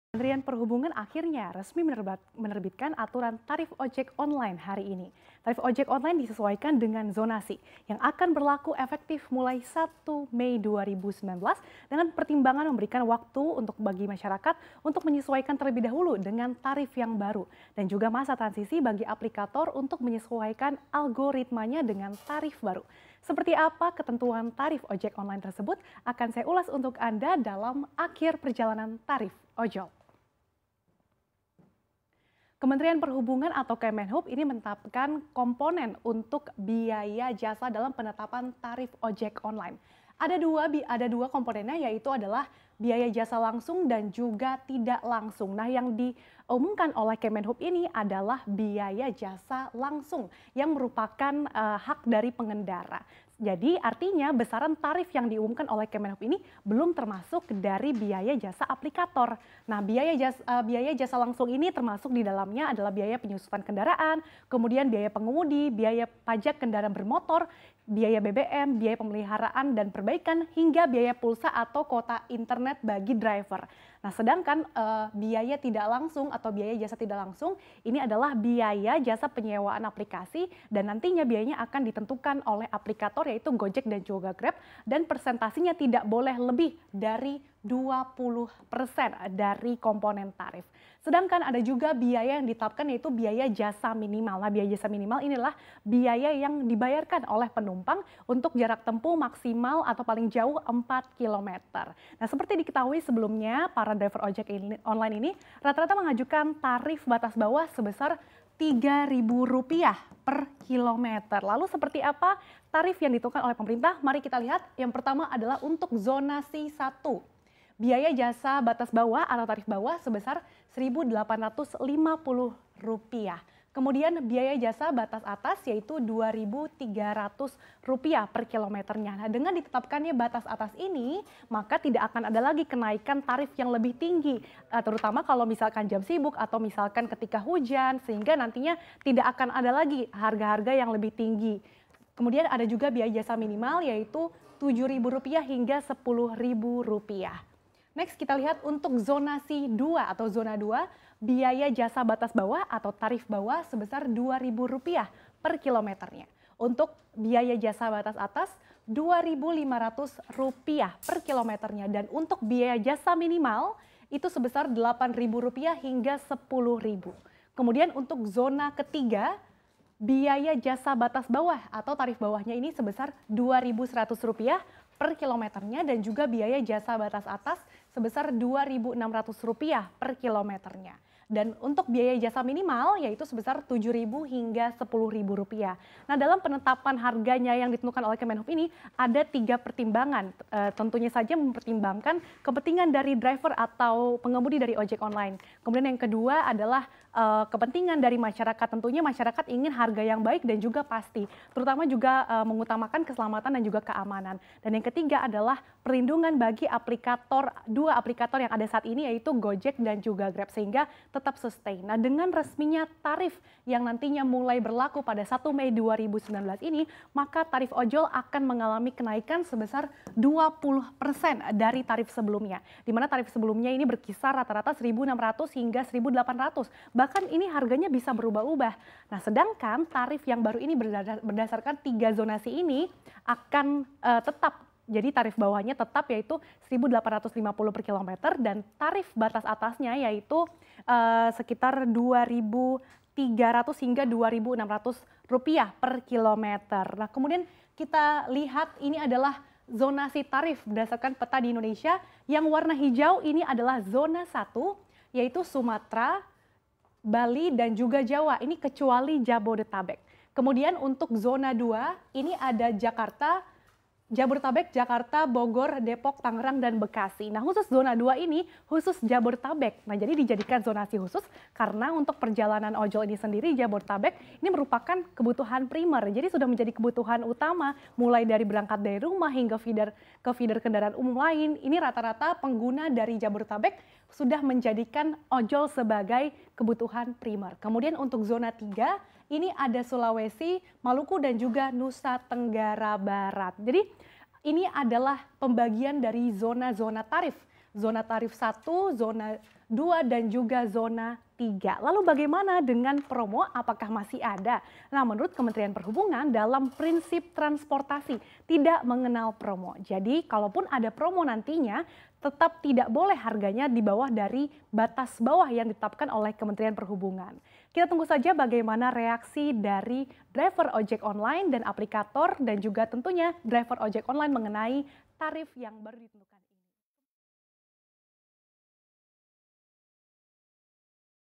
Akhir Penantian Tarif Ojol
Kementerian Perhubungan akhirnya resmi menerbitkan aturan tarif ojek online hari ini. tarif ojek online disesuaikan dengan zonasi yang akan berlaku efektif mulai 1 mei 2019. Dengan pertimbangan memberikan waktu masyarakat untuk menyesuaikan dengan tarif yang baru dan masa transisi bagi  aplikator untuk menyesuaikan algoritmanya dengan tarif baru.

Like & Follow Facebook:
CNBC Indonesia

cnbcindonesia